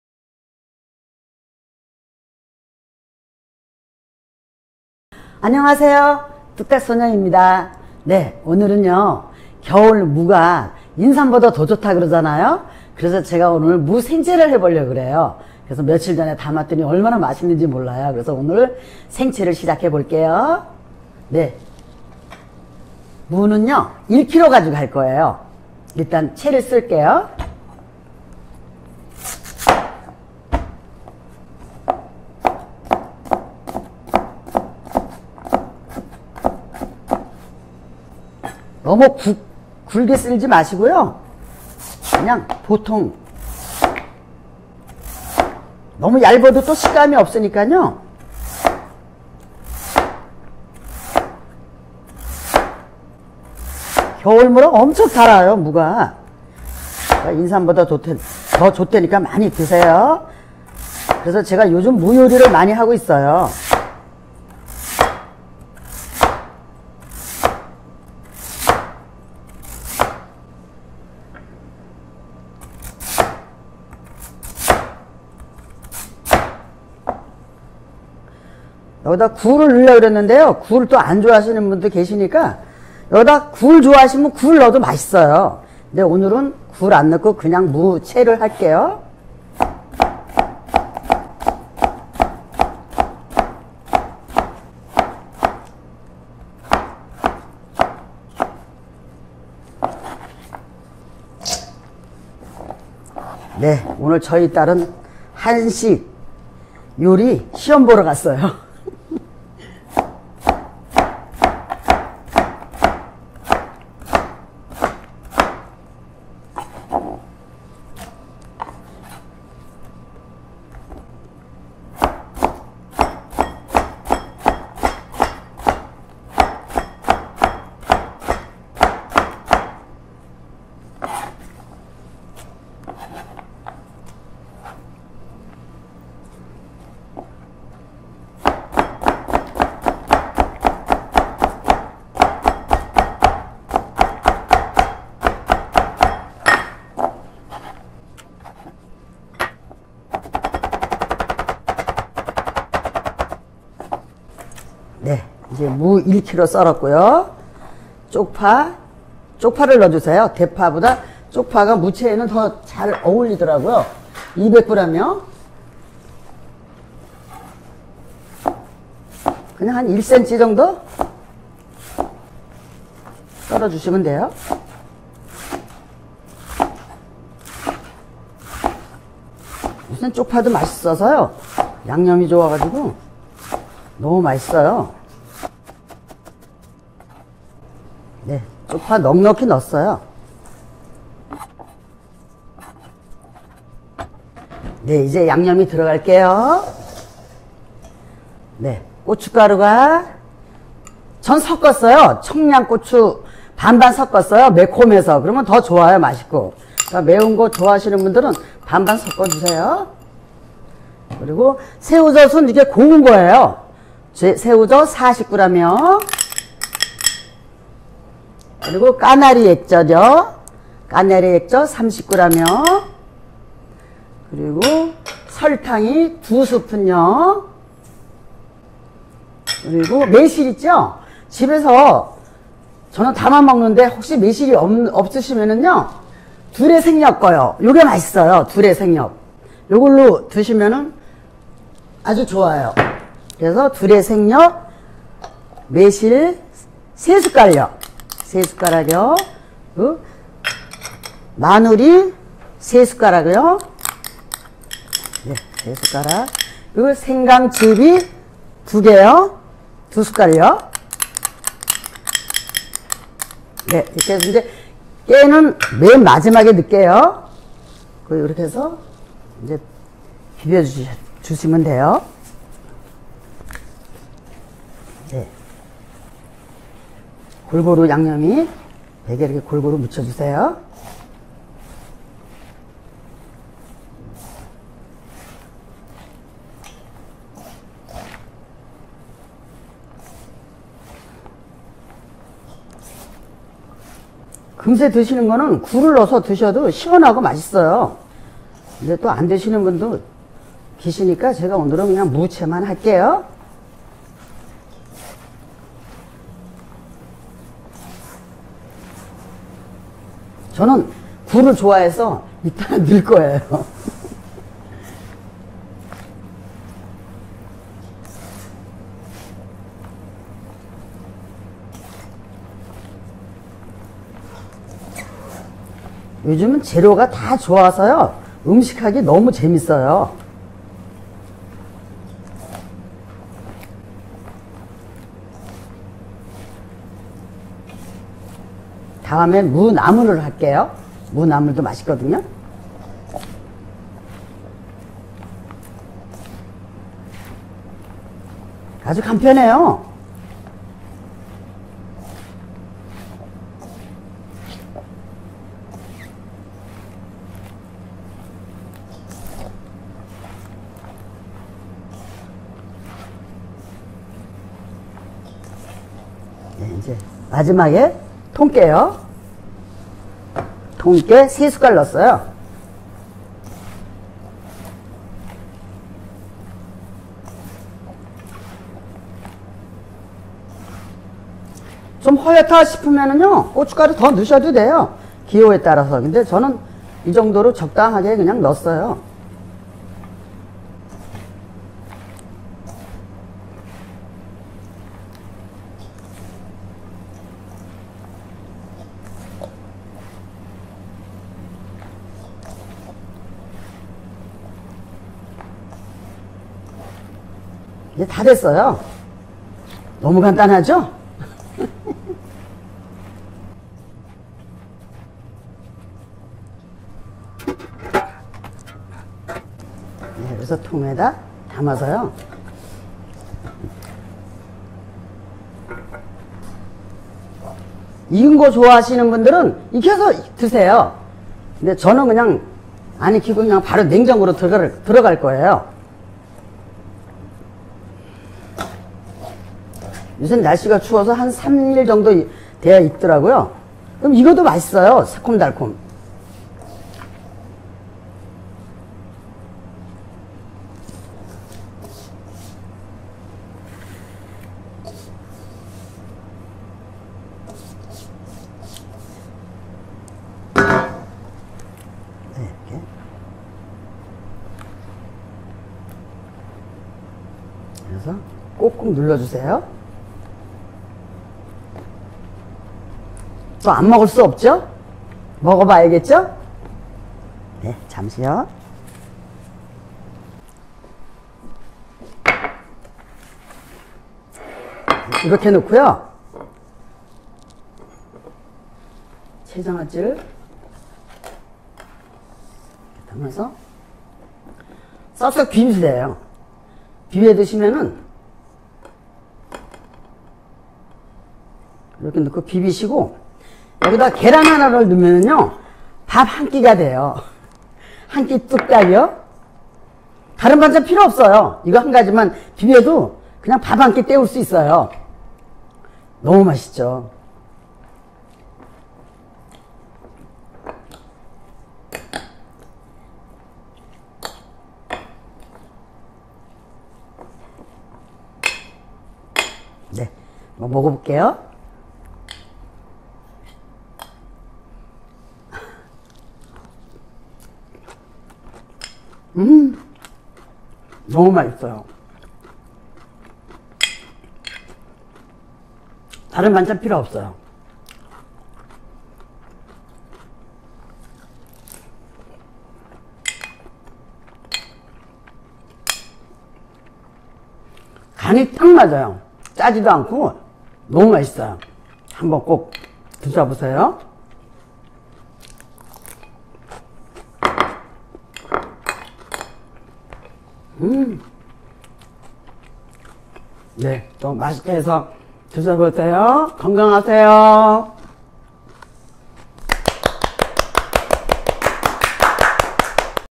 겨울보약 무 ' 무생채 '
겨울보약 무로 무생채를 만들어 보았습니다.
무 하나에 집에 있는 재료로 손쉽게 만들어 보세요.